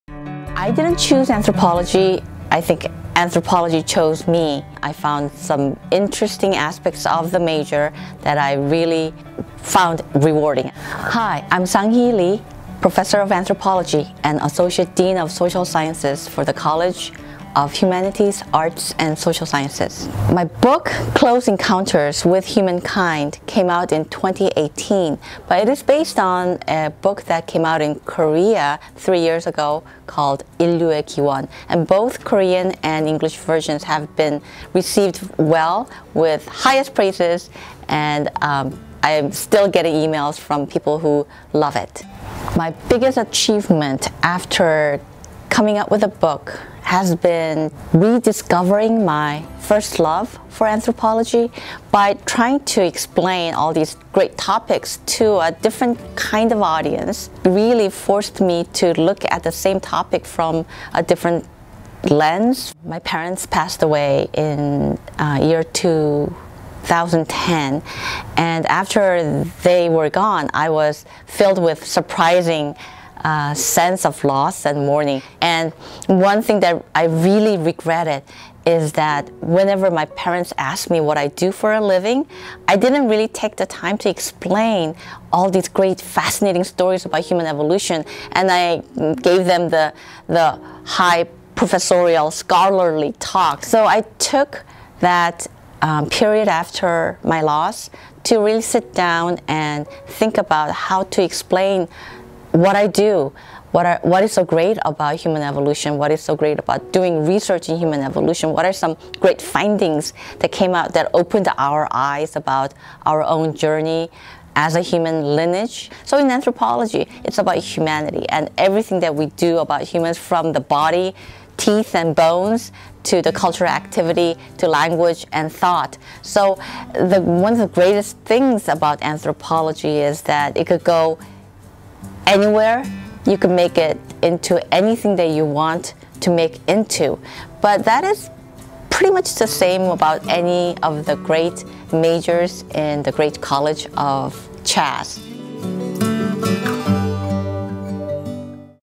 Anthropology Professor and Author: Sang-Hee Lee Overview video
After winning multiple awards on her new book, anthropology professor and now award winning author, Sang-Hee Lee explains her journey.